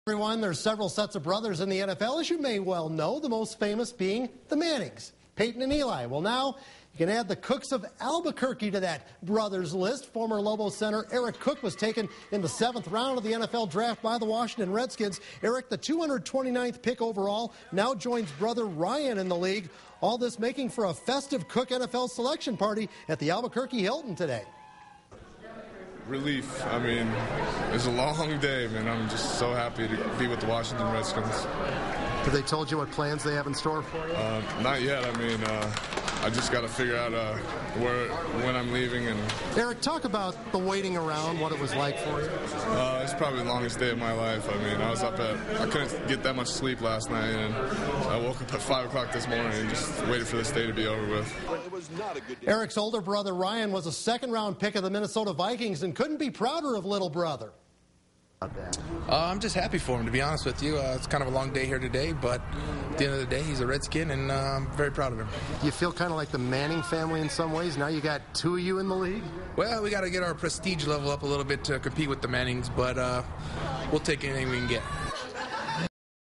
Ex-Lobo Erik Cook picked in NFL Draft
ALBUQUERQUE (KRQE) - With the 22nd pick in the seventh round of the 2010 NFL Draft, the Washington Redskins selected former New Mexico Lobo Erik Cook.